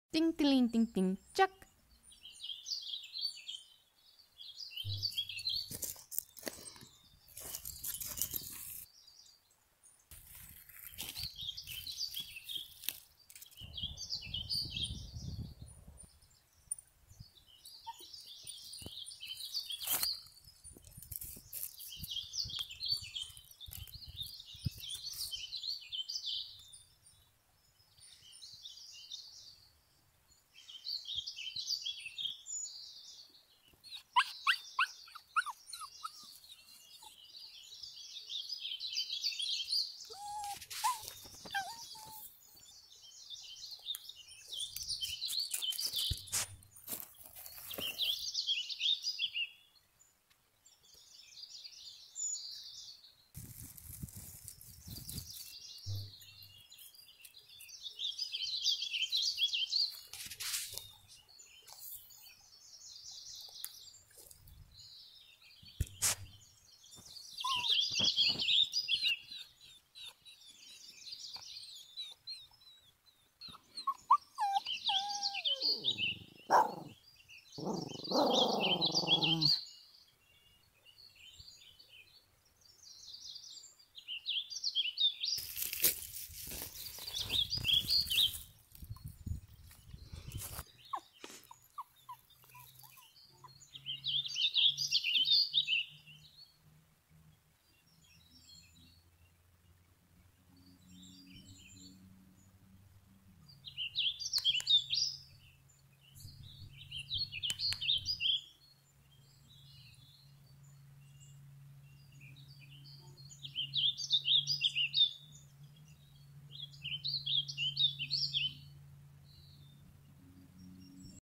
Pomeranian vs Squirrel
Watch Chuck as he is search for his nemesis, the squirrel, in this funny episode. This Pomeranian is constantly being taunted by his enemy and Chuck will travel far and wide to put a stop to him.